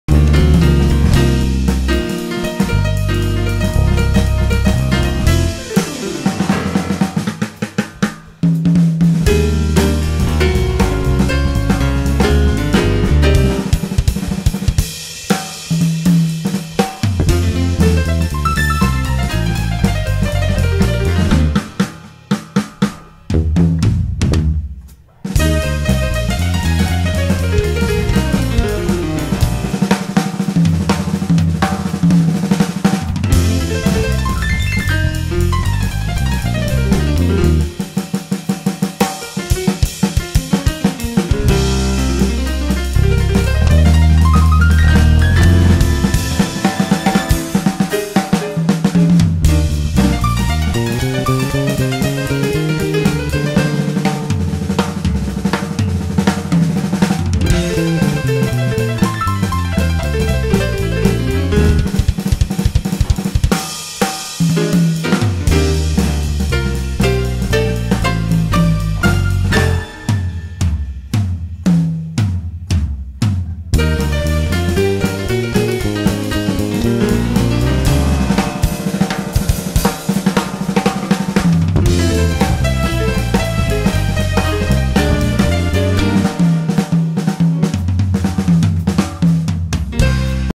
Emmanuelle Caplette on drum: 4 bars Solo (Nov. 2009)
In a Jazz Club! Jam night! 4 bars drum solo. A lot of fun!!!

Sylvain Bertrand: Bass
Emmanuelle Caplette: Drum